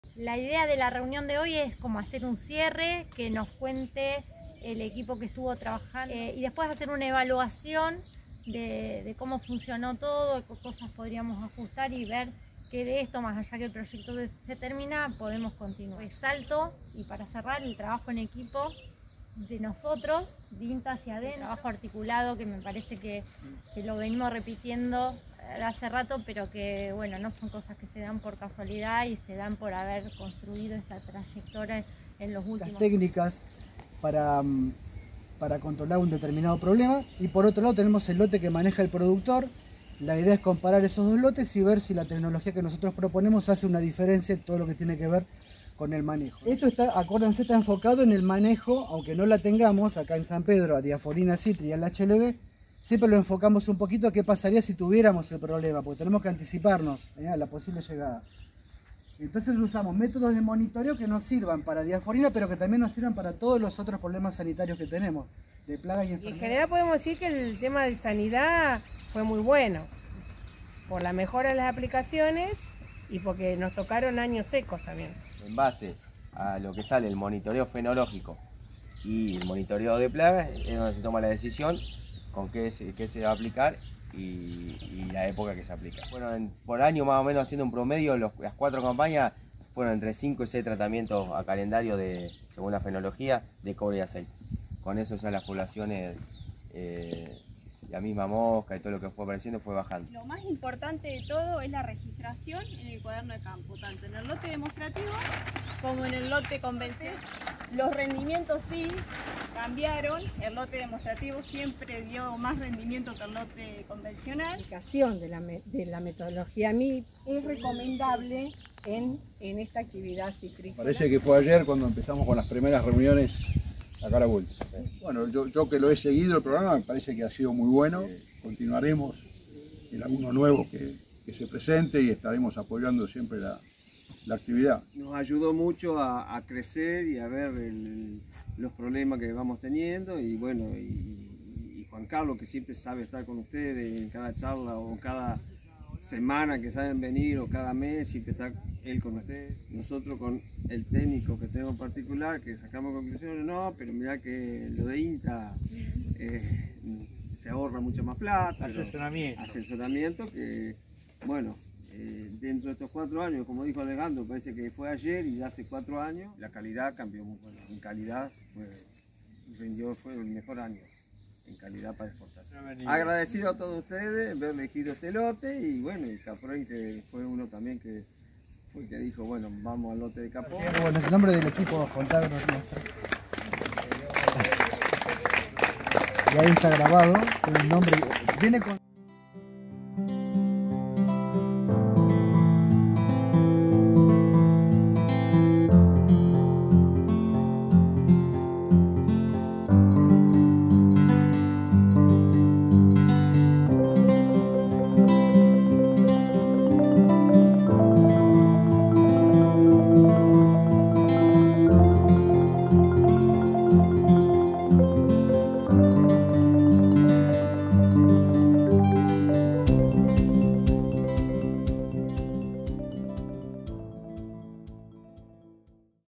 FONTAGRO taller fin de año 22 nov 2023 // Versión Resumida
Después de 4 años de trabajo, se realizó el taller de cierre con la presentación de algunos resultados que dan cuenta del proceso de las actividades realizadas en el lote del productor Juan Carlos Capó y en el marco del Proyecto "Control del vector del HLB en la Agricultura Familiar de Argentina, Uruguay, Paraguay y Bolivia".